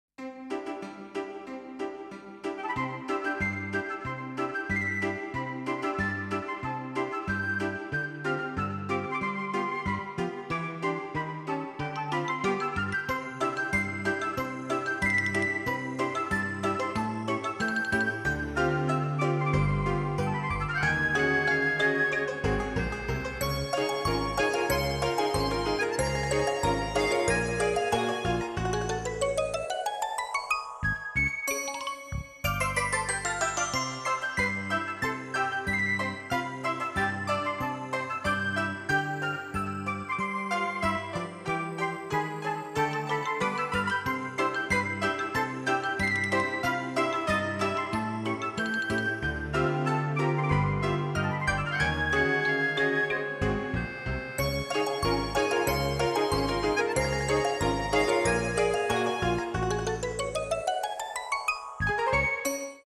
N Hayat... Ayıların havuz keyfi!
Sıcaktan bunalan ayılar çareyi havuzda serinlemekte buldu.